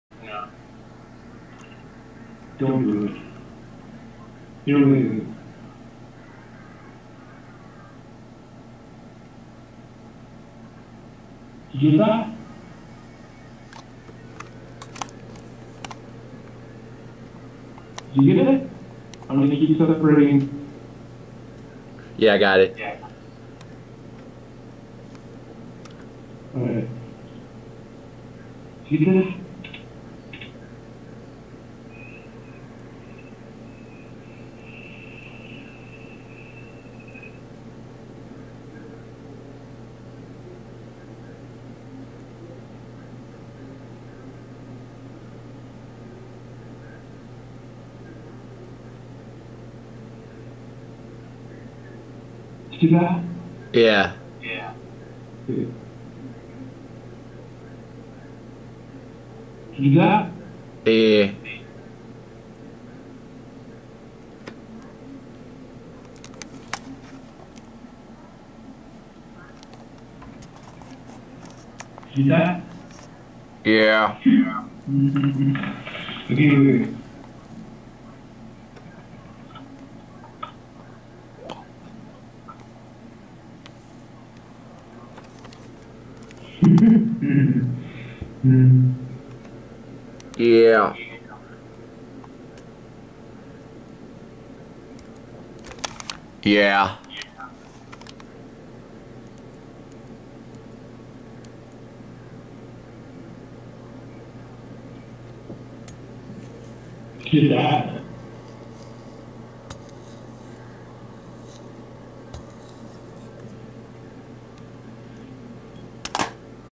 BULB (Episode 586)
John watches Kurt give the horns sign.